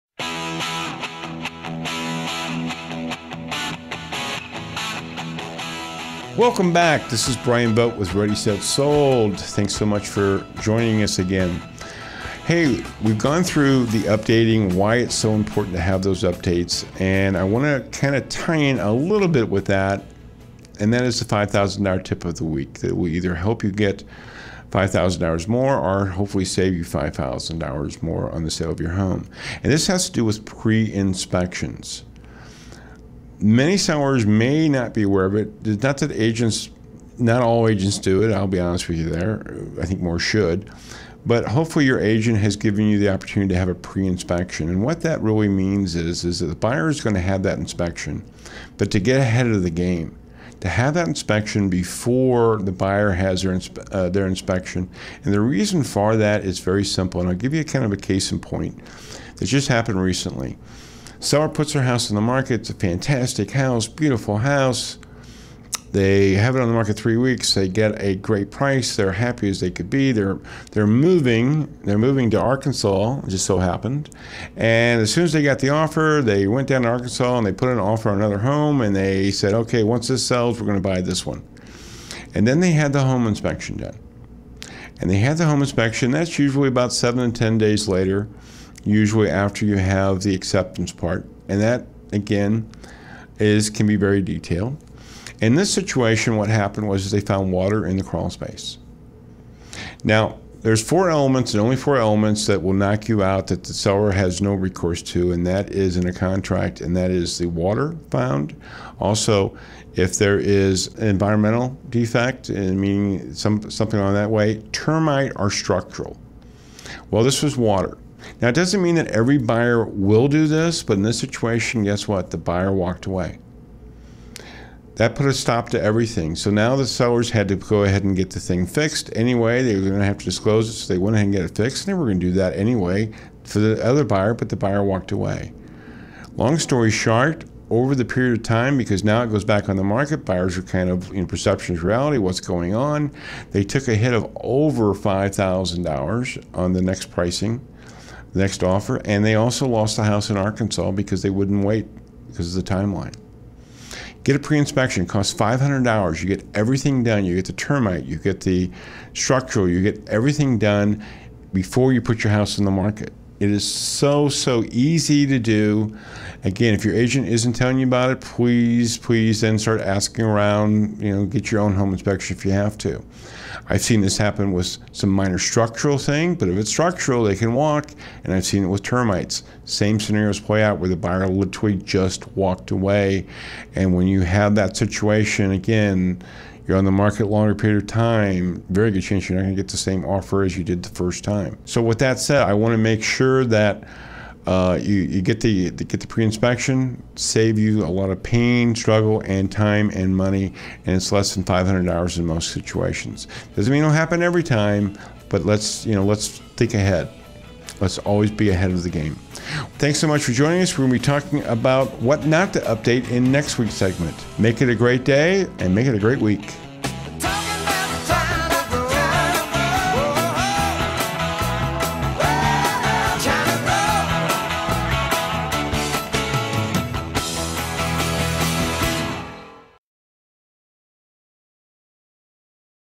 Ready Set Sold with Bryan Vogt #13-05: $5000 tip of the week: The value of pre-inspections
Bryan Vogt: Welcome back, this is Bryan Vogt with Ready, Set, Sold. Thanks so much for joining us again. Hey, we've gone through the updating, why it's so important to have those updates. And I want to kind of tie in a little bit with that, and that is the $5,000 tip of the week that we either help you get $5,000 more, or hopefully save you $5,000 more on the sale of your home.

This has to do with pre-inspections. Many sellers may not be aware of it, not all agents do it, I'll be honest with you there, I think more should, but hopefully your agent has given you the opportunity to have a pre-inspection. And what they really means is, the buyer's going to have that inspection. But to get ahead of the game, to have that inspection before the buyer has their inspection. And the reason for that, is very simple, and I'll give you kind of a case in point. It just happened recently.

Someone puts their house on the market. It's a fantastic house, beautiful house. They have it on the market three weeks, they get a great price. They're happy as they could be. They're moving to Arkansas, just so happened, and as soon as they got the offer, they went down to Arkansas and they put an offer on another home, and they said, "Okay once this sells, we're going to buy this one."

And then they had the home inspection done. And they had the home inspection, that's usually about 7 and 10 days later, usually after you have the acceptance part. And that, again, can be very detailed. In this situation what happened, was they found water in the crawl space. Now, there's four elements, and only four elements that will knock you out that the seller has no recourse to, and that is in a contract, and that is the water found; also, if there is environmental defect, meaning something along that way; termite, or structural.

Well, this was water. Now, doesn't mean that every buyer will do this, but in this situation, guess what, the buyer walked away. That put a stop to everything. So, now the sellers had to go ahead and get the thing fixed anyway. They were going to have disclose. They went ahead and got it fixed. They were going to do that anyway for the other buyer, but the buyer walked away. Long story short, over the period of time, because now it goes back on the market, buyer’s perception are, "What's going on?"

They took a hit of over $5,000 on the next pricing, the next offer. And they also lost the house in Arkansas, because they wouldn't wait, because of the timeline. Get a pre-inspection, costs $500. You get everything done. You get the termite. You get the structural. You get everything done before you put your house on the market. It is so, so easy to do. Again, if your agent isn't telling you about it, please, please insert asking around, you know, get your own home inspection if you have to.

I've seen this happen with some minor structural thing. But if it's structural, they can walk, and I've seen it with termites. Same scenarios play out with a buyer would tweet, "Just walked away." And when you have that situation, again, you're on the market longer period of time, very good chance you're not going to get the same offer as you did the first time.

So, with that said, I want to make that you get the pre-inspection. Save you a lot of pain, struggle, and time, and money, and it's less than $500 in most situations. This doesn't mean it'll happen every time, but let's, you know, let's think ahead. Let's always be ahead of the game. Thanks so much for joining us. We'll be talking about what not to update in next week’s segments. Make it a great day. And make it a great week.